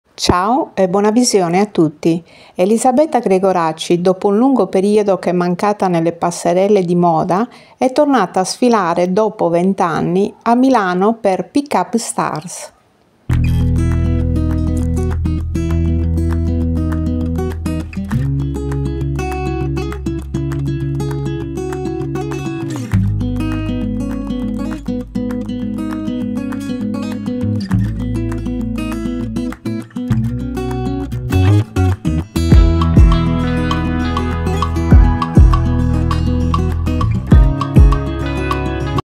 Elisabetta Gregoraci in passerella a Milano sfila per Pin Up Stars video fashion news 22/09/2024
Elisabetta Gregoraci, sfila alla Milano Fashion Week 2024 dopo vent'anni per Pin Up Stars.